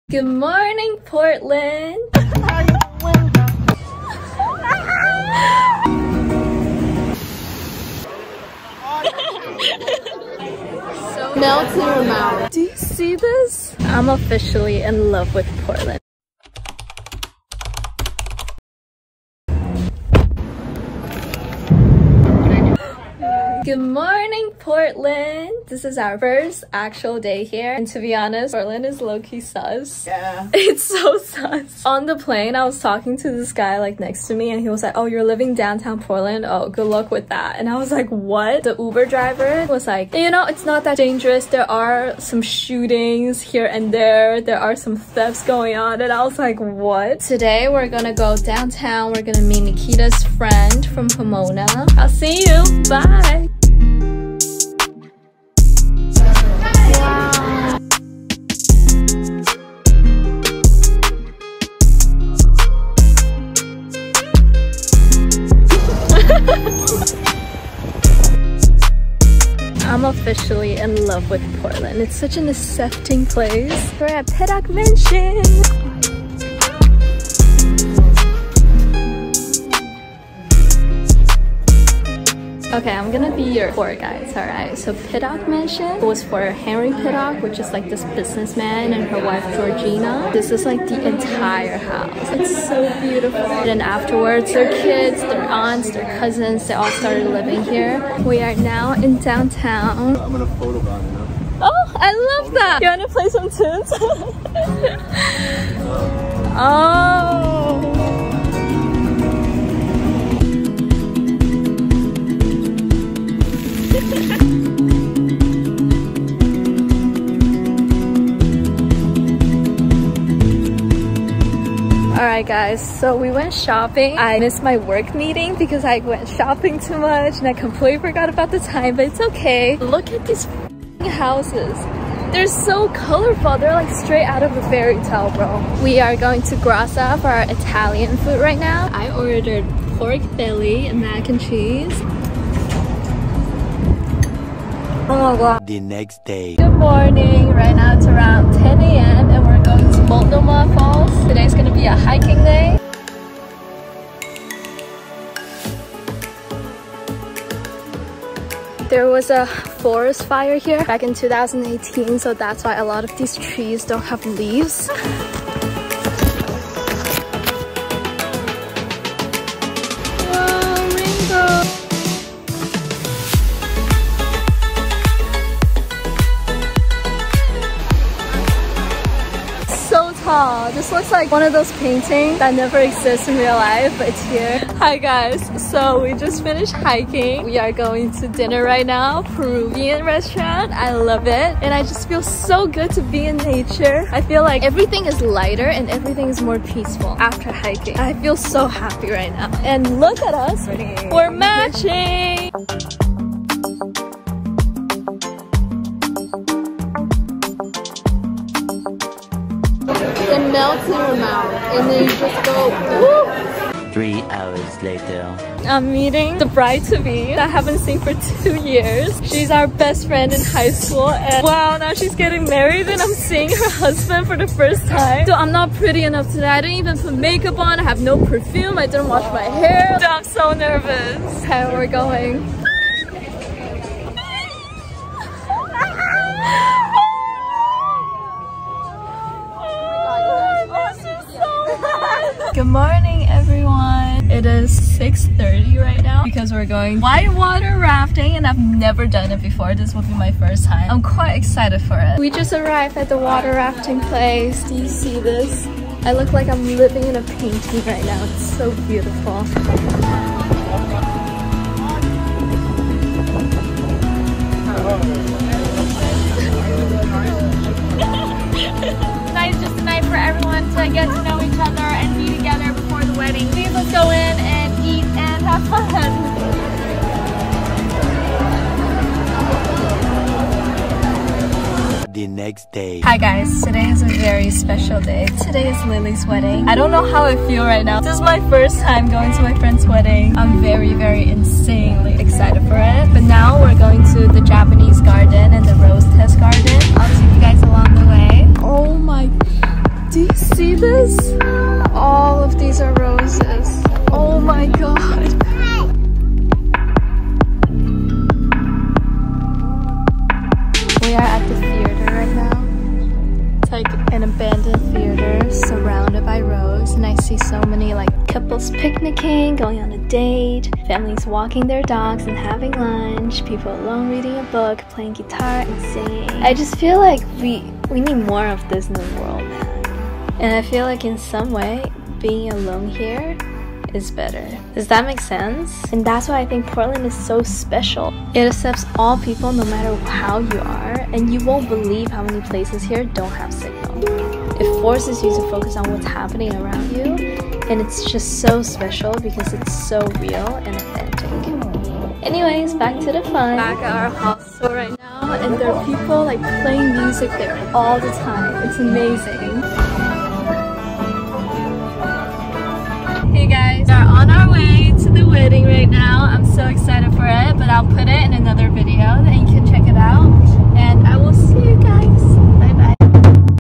PORTLAND TRAVEL VLOG/GUIDE | A week in Portland
My best friend since high school had her wedding in Portland!
Must-gos:
1. Washington Park (International Rose Test Garden and Japanese Garden)
2. Pioneer square and walk around downtown
3. Multnomah Falls (try the 3-hr Wahkeena Falls trail to see 5+ waterfalls!)
4. Beer and other alcoholic drinks there are amazing! Very 420 friendly too
5. Shopping: ALL TAX-FREE! Ball out!

Special things about Portland:
1. SO many rose gardens and parks
2. People are so accepting, extremely LGBTQ+ community friendly
3. Very spiritual and friendly community, you can start a conversation with anyone on the streets
4. However, they do have a serious homeless problem due to drugs and inefficient political support to the issue :(